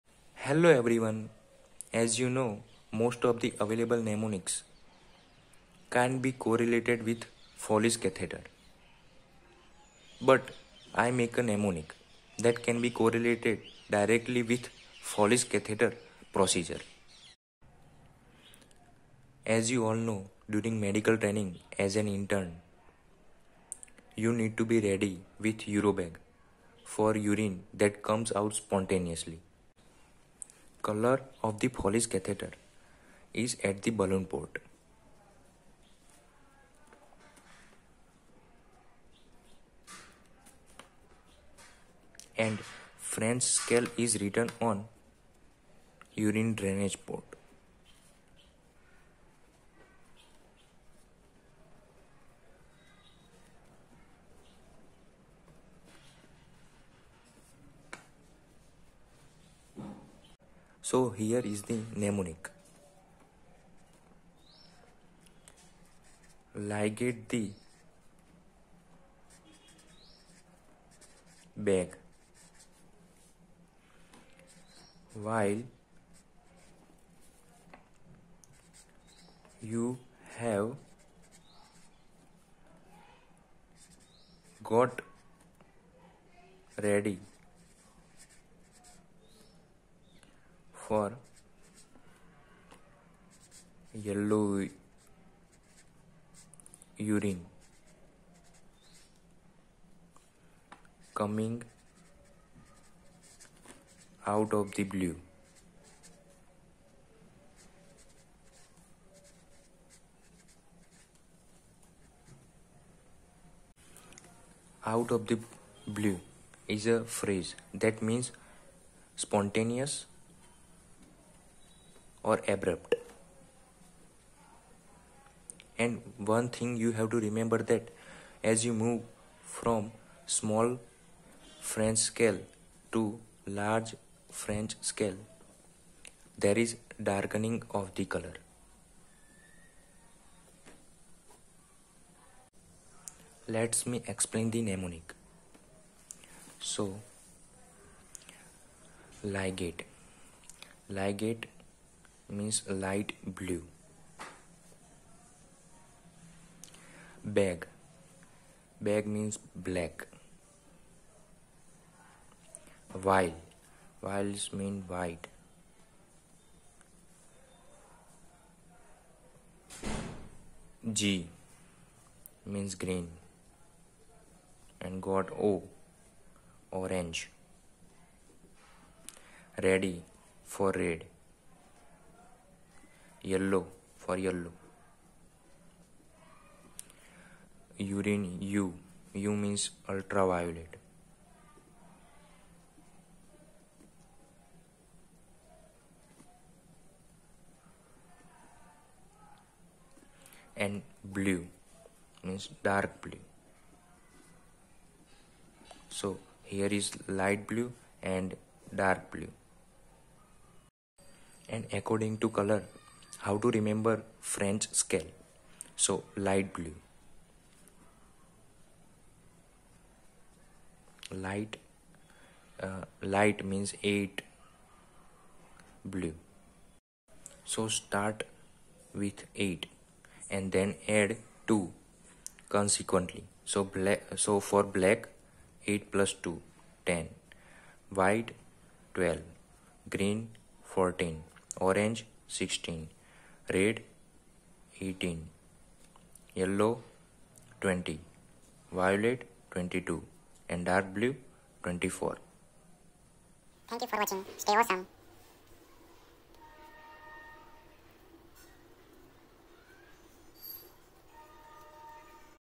Mnemonic for Foley’s Catheter: French scale & Color code
Video by Drashtant.
For NEET PG , AIIMS entrance exams.